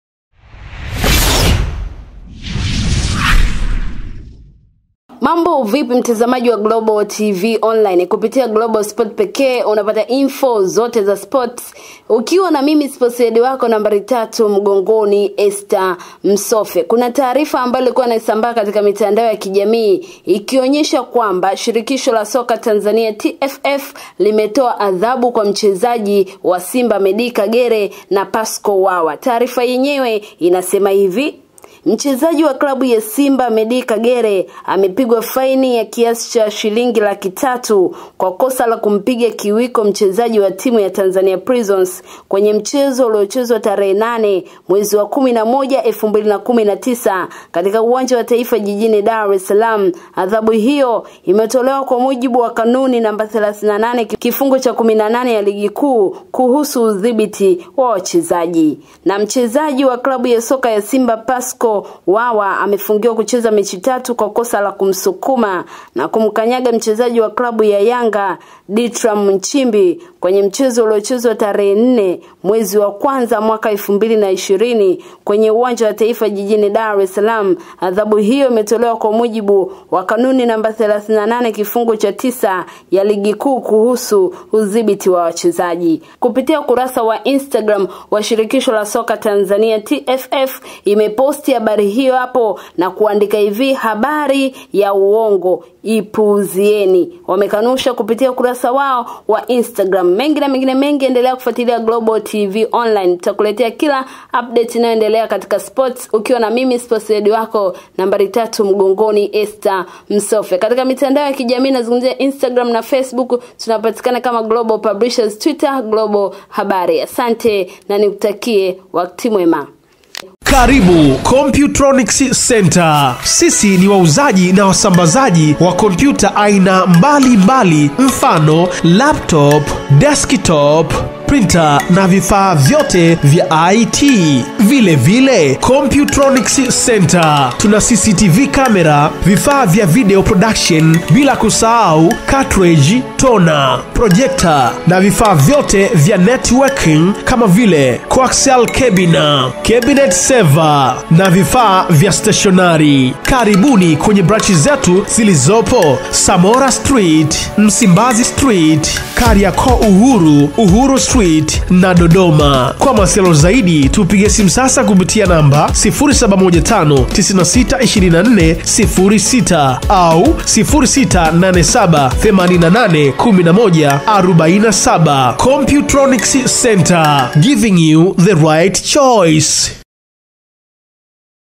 ISHU ya KAGERE KUTOZWA FAINI, WAWA KUFUNGIWA, TFF WAFUNGUKA..
Shirikisho la Soka Tanzania TFF limekanusha taarifa ambazo zinaendelea kusambaa katika mitandao ya kijamii kuwa limemtoza faini ya Shilingi laki tano mchezaji wa Simba Meddie Kagere na kumfungia mechi tatu Pascal Wawa.

UNA HABARI? WASILIANA NA GLOBAL TV ONLINE